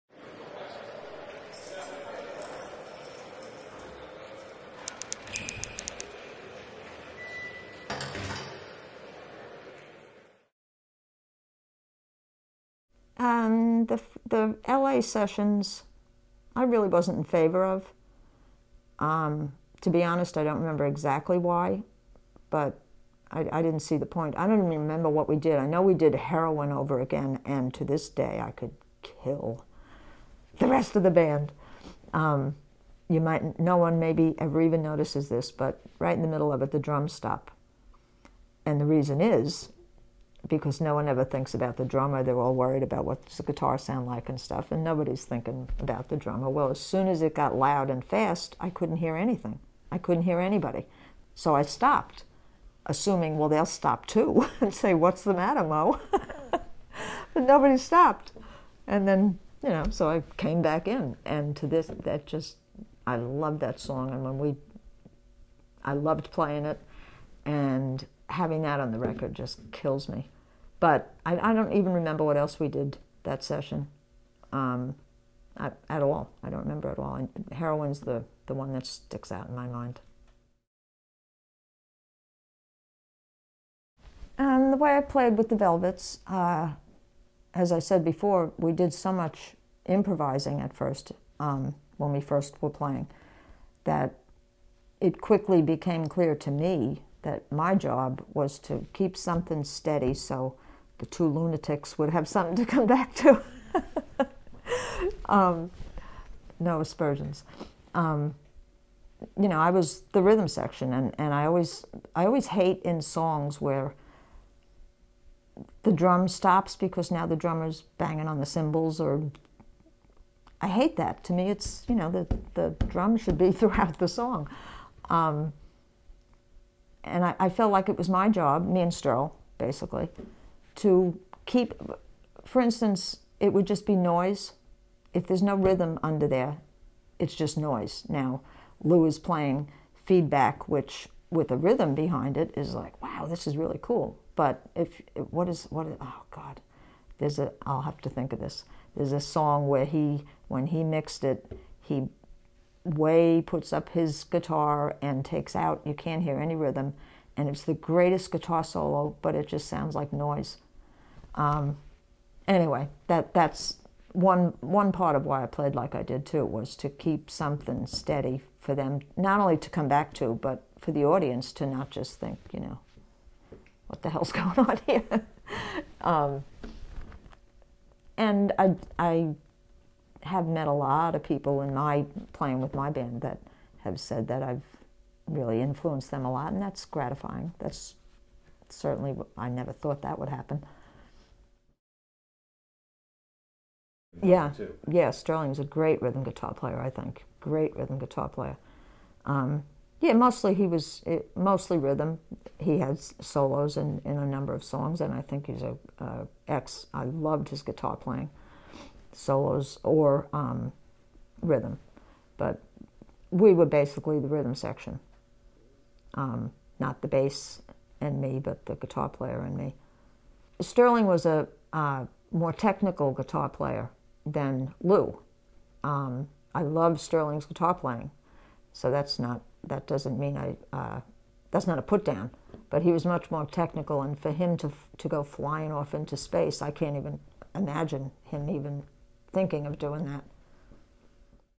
The Velvet Underground: Moe Tucker Part 5
Maureen "Moe" Tucker is a musician best known for having been the drummer for the rock group The Velvet Underground. In this interview Moe looks back at The Velvets' history and her own role in one of modern music's most innovative and influential bands.